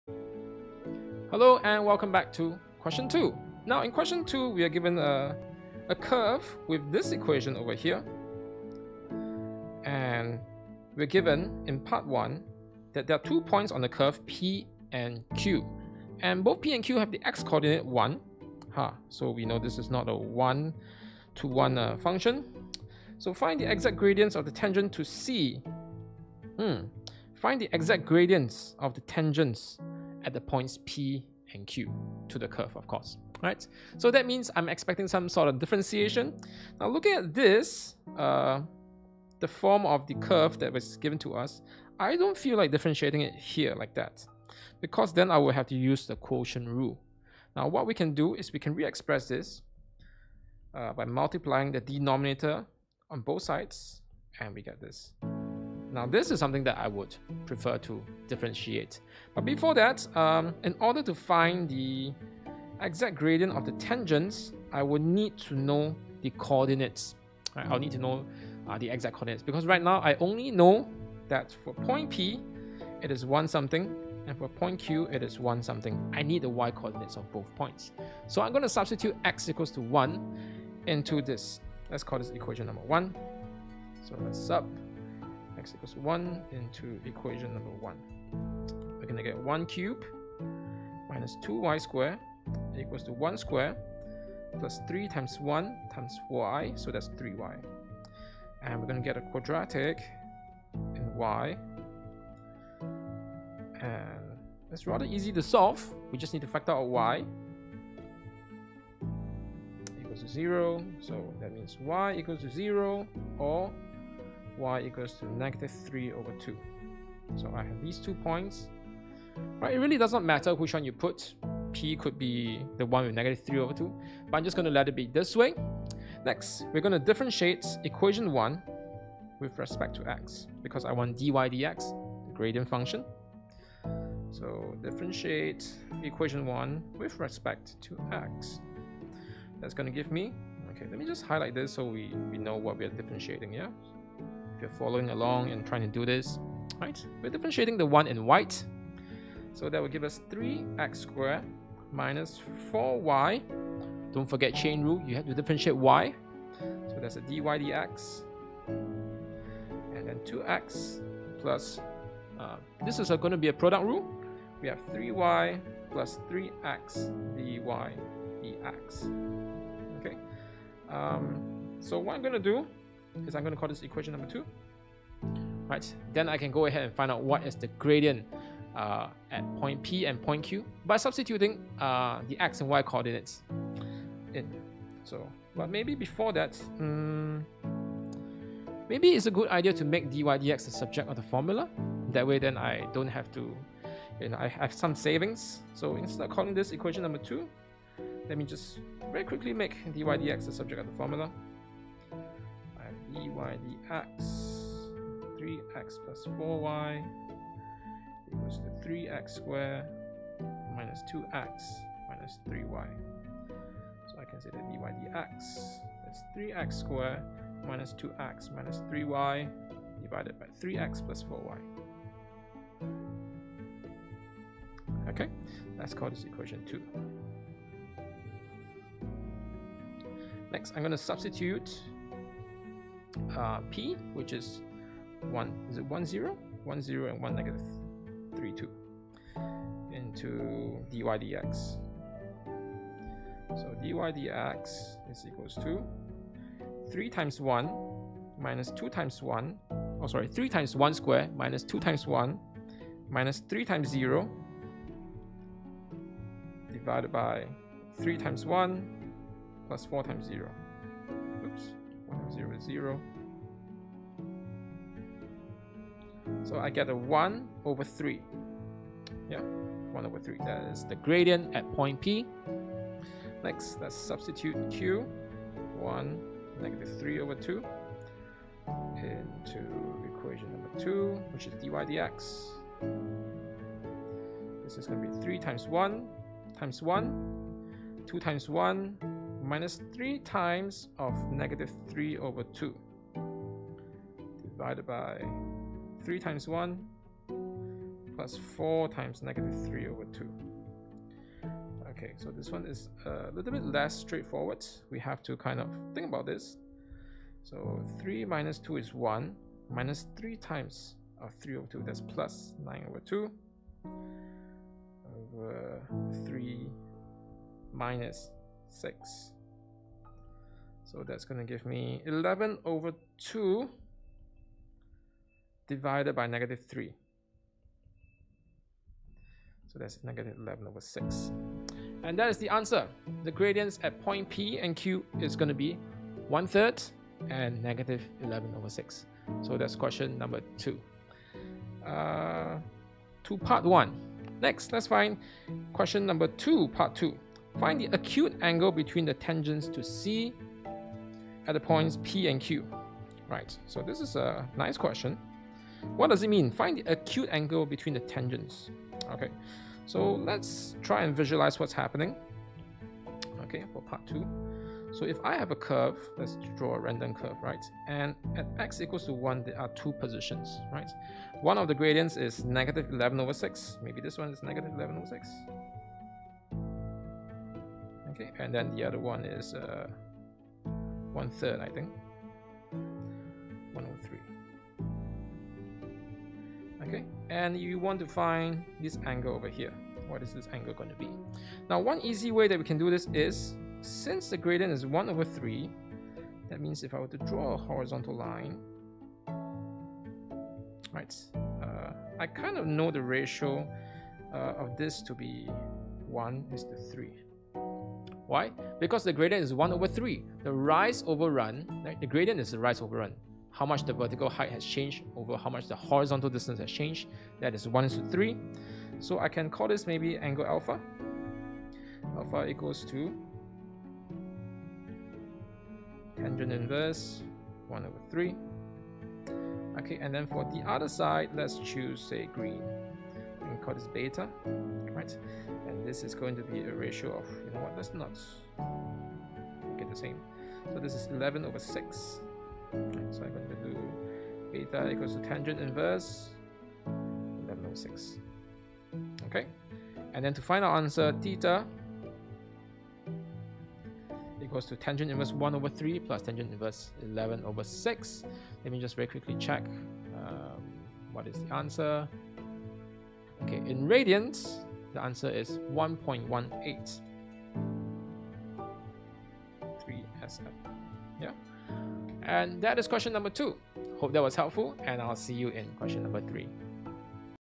Singapore A Levels H2 Maths 9758 - ACJC 2021 Prelim P1 Q2
Uh-Oh! It's that time of the year where everyone is scrambling to flip through the old lecture notes again!

Let's revise together
- Differentiation